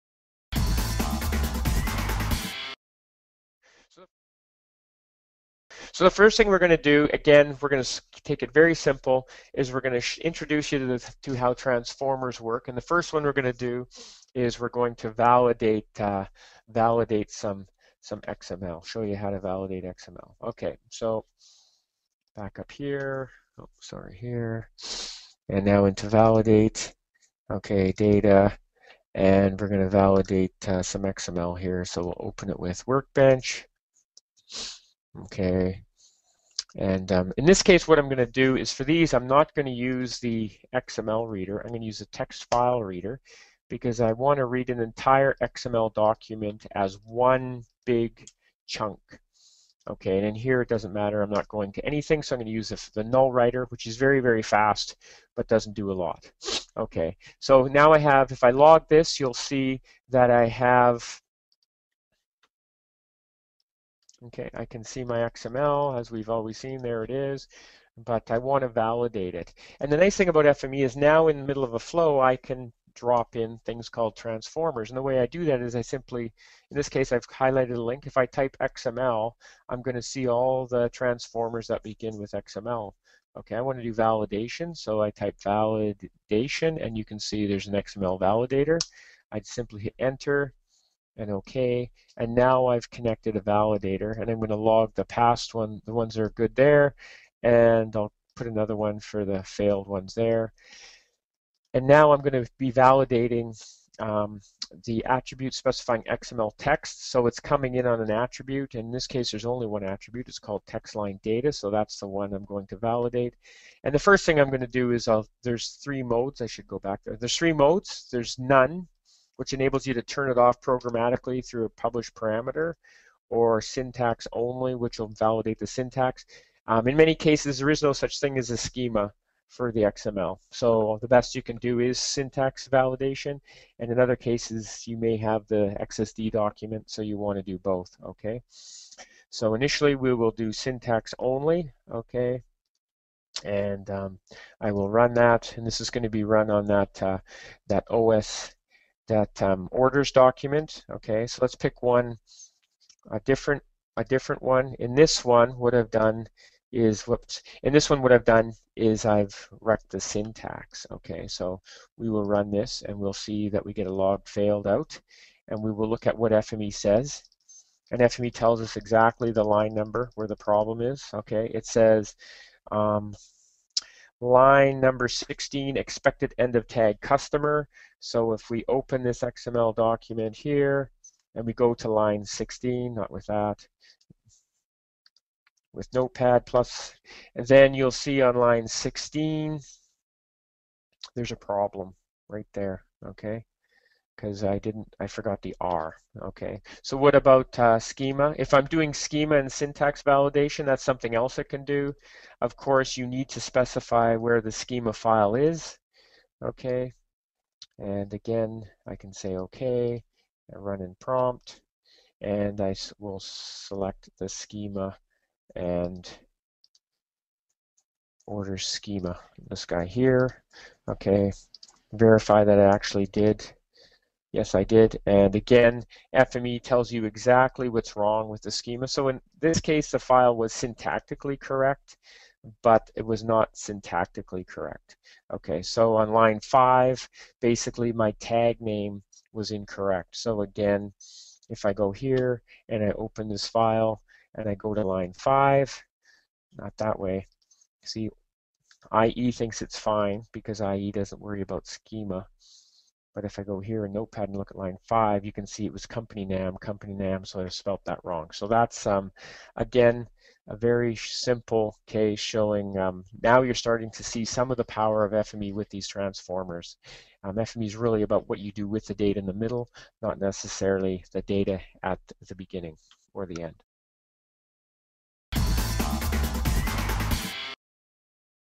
Validate XML syntax and schema
In this demo you'll see how to use FME transformers to validate any XML data. We'll first read in an XML file as blob text, then insert the XMLValidator transformer to validate the syntax and schema. The output log file will tell us where the input file contains an error.